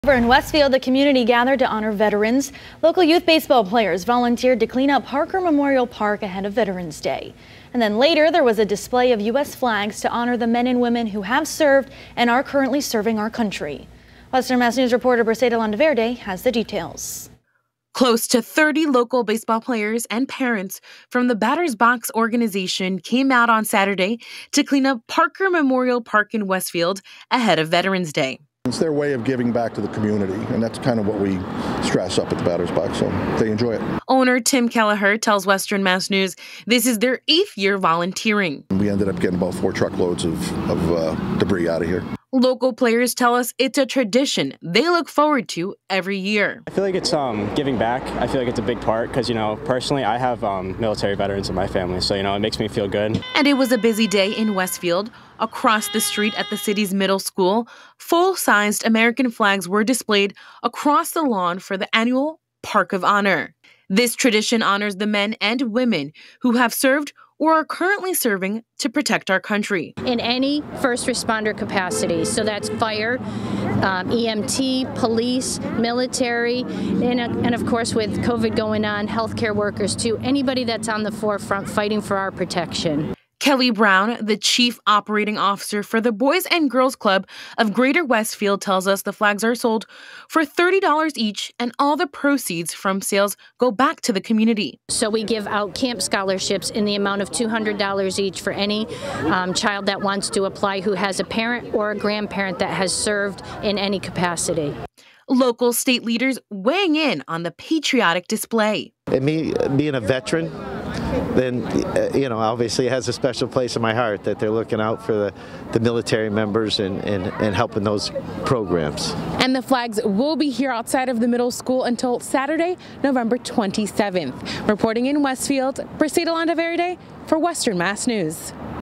Local youth baseball players honor veterans with park cleanup, Park of Honor
Local youth baseball players volunteered to clean up Parker Memorial Park ahead of Veterans Day. Later, there was a display of U.S. flags to honor the men and women who have served and are currently serving.